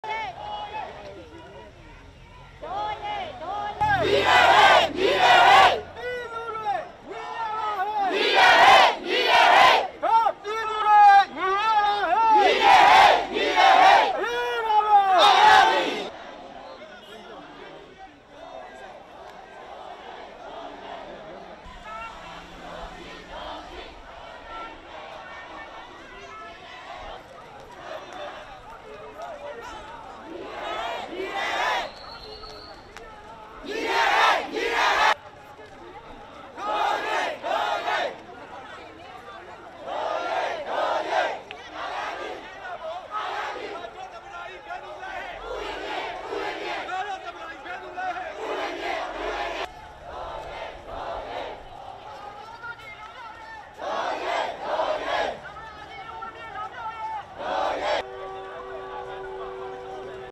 Myanmar: Protesters gather at NLD office in Yangon and march
Anti-coup protesters defy a new military ban on rallies outside the NLD office in Yangon before heading to Sule pagoda for a fourth straight day of street demonstrations.